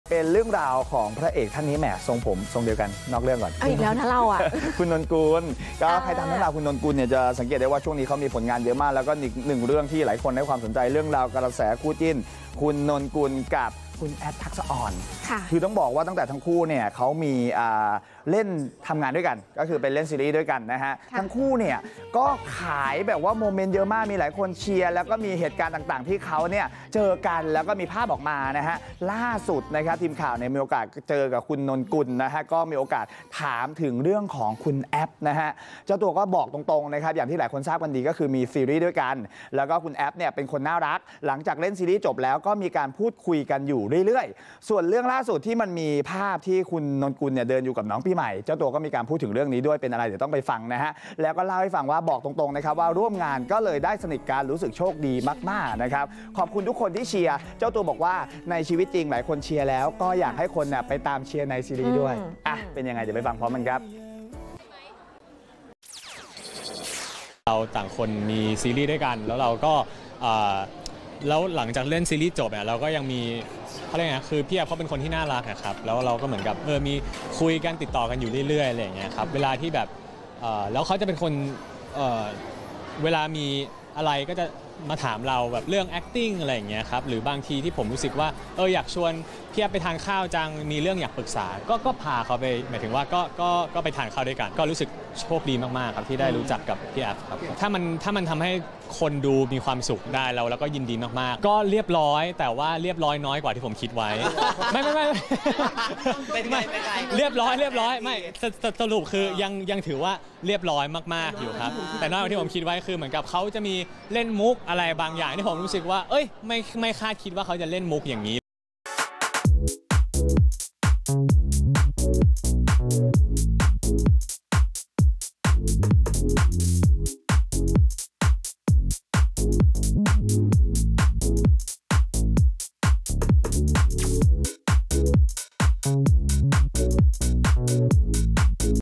“นนกุล” ไฟเขียวแฟนคลับจิ้น “แอฟ ทักษอร” บอกโชคดีที่ได้รู้จัก | Apop Today
นักแสดงหนุ่มมากความสามารถ “นนกุล” เป็นอีกคนที่แฟนๆ จับจิ้นกับนางเอกสาวรุ่นพี่อย่าง “แอฟ ทักษอร” ด้วยความน่ารักของทั้งคู่ หลังจากมีภาพไปดูละครเวทีด้วยกัน ก็ยิ่งตอกย้ำกระแสจิ้นไปอีก ซึ่งด้าน “นนกุล” ก็ได้เผยถึงความประทับใจ พร้อมเล่ามุมที่หลายคนไม่เคยเห็นจาก “แอฟ ทักษอร”